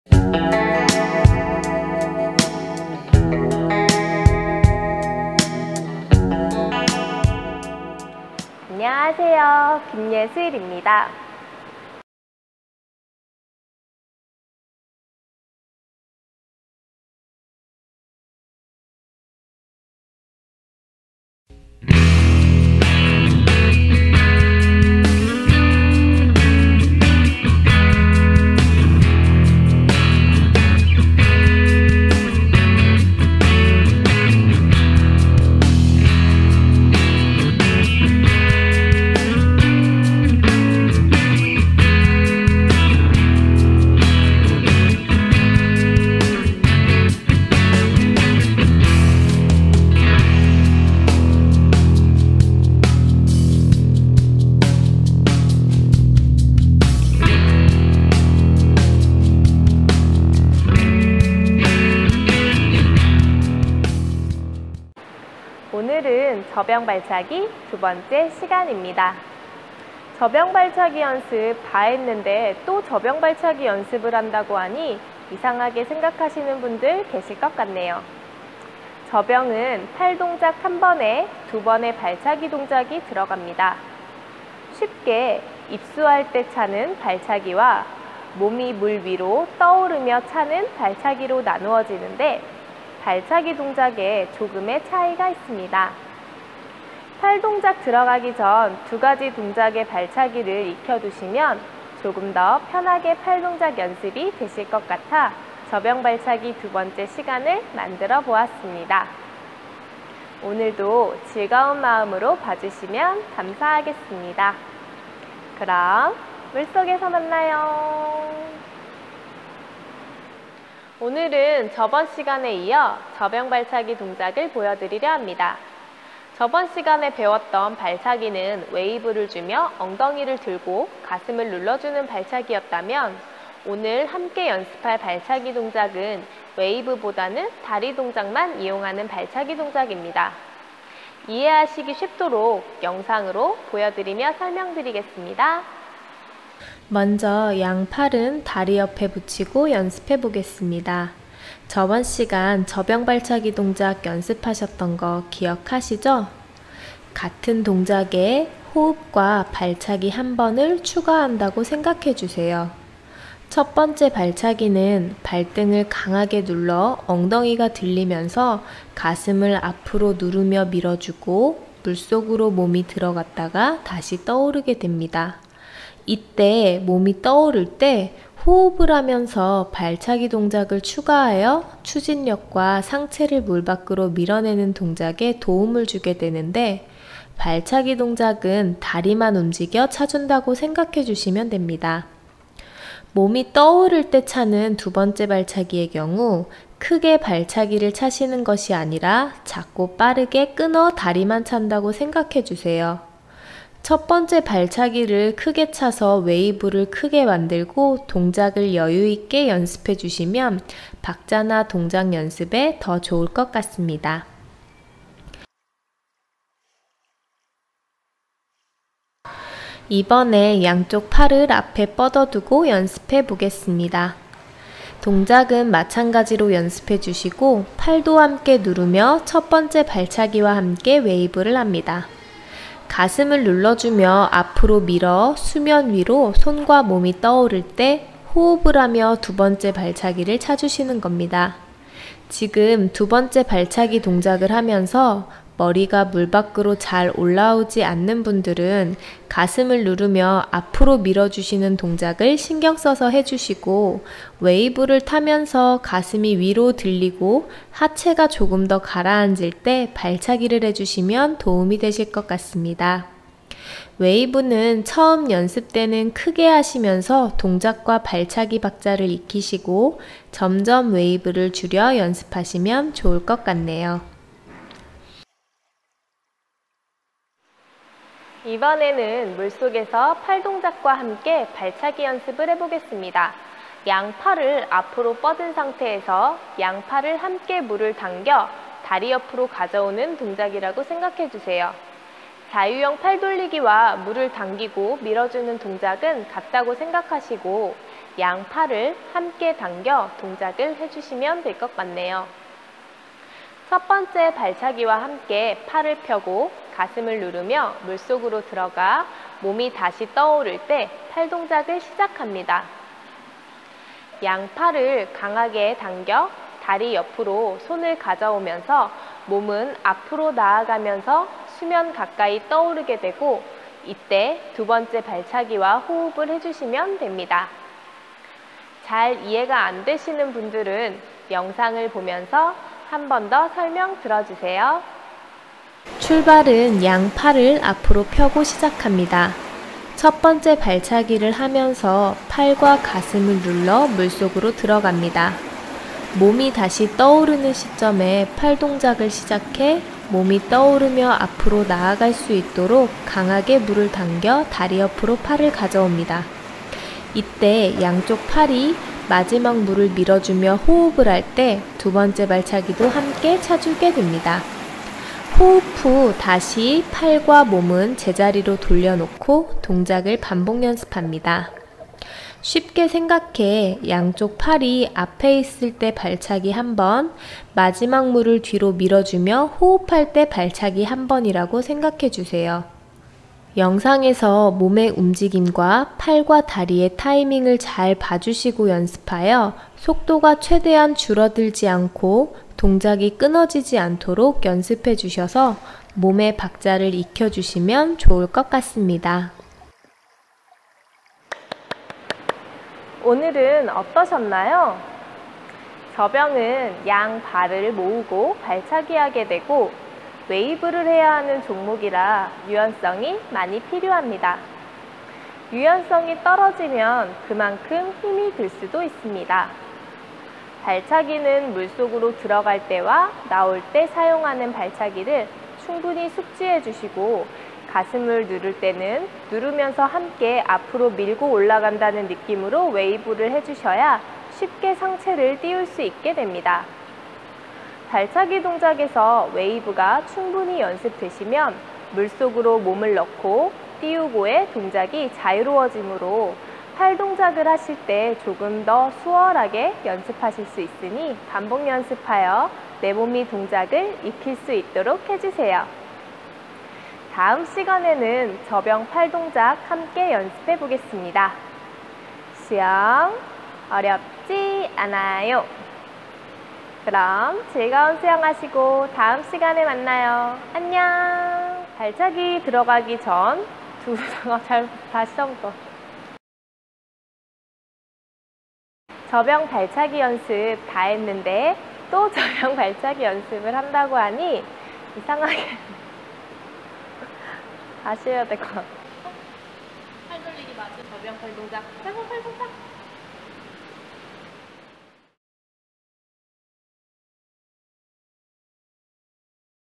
(한국어,Eng cc) 수영강습 접영편 #14 (국가대표 수영선수 김예슬) 접영 발차기 Butterfly kicking PART. 2/2
Hello there! This is Yesul.
We also upload the videos on NAVER TV, so go and check it out of NAVER TV as well!